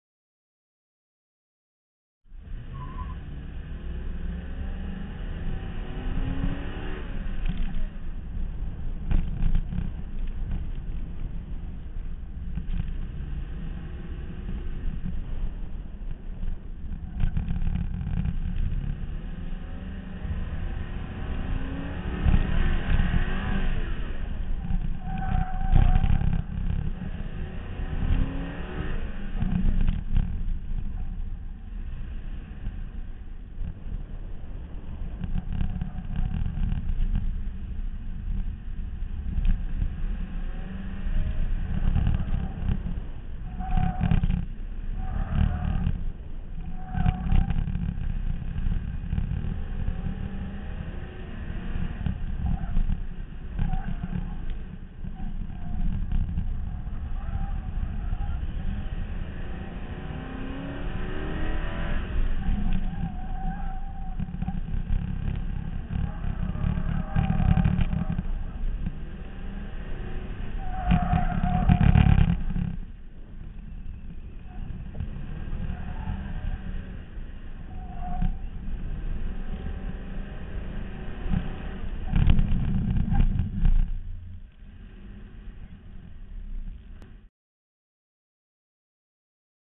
5_5_2013 Willamette Motor Club Autocross Run 3
Racing at the Oregon State Fairgrounds in a bone-stock 2001 Cadillac Seville with Yokohama touring tires.